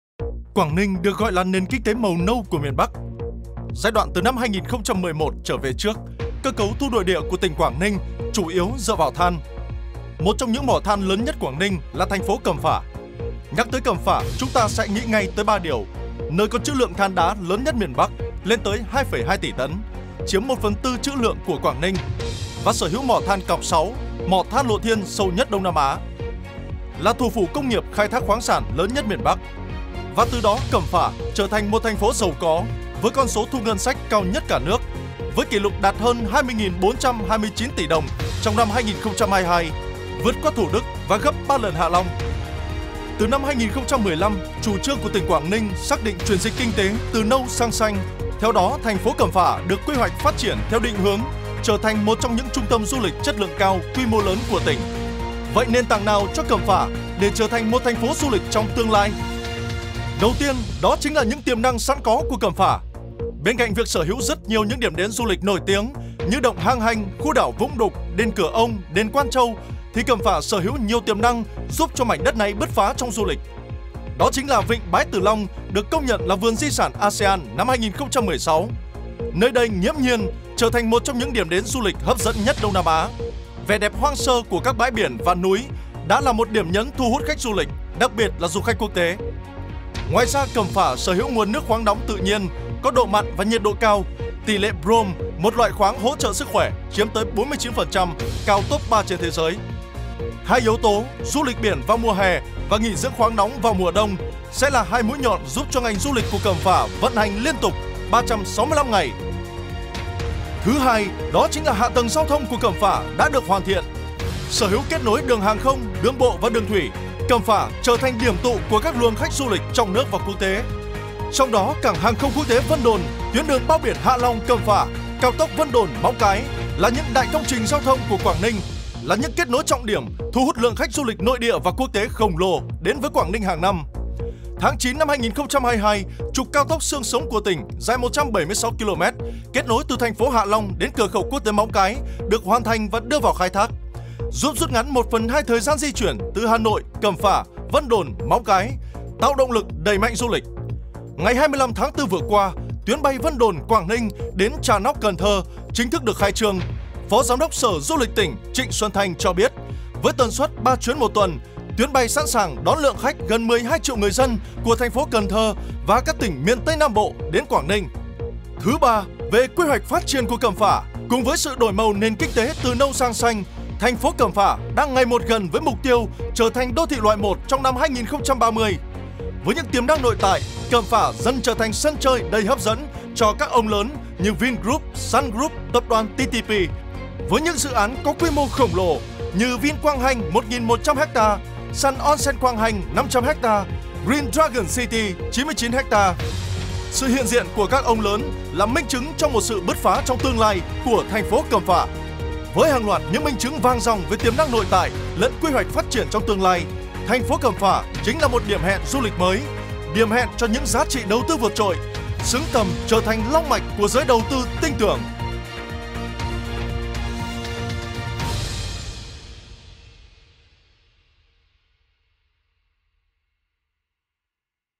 Cẩm Phả - Điểm hẹn du lịch mới của Vịnh Bắc Bộ
Với hàng loạt những minh chứng vàng ròng về tiềm năng nội tại lẫn quy hoạch phát triển trong tương lai, thành phổ Cẩm Phả chính là một điểm hẹn du lịch mới, điểm hẹn cho những giá trị đầu tư vượt trội, xứng tầm trở thành “long mạch” của giới đầu tư tinh tường!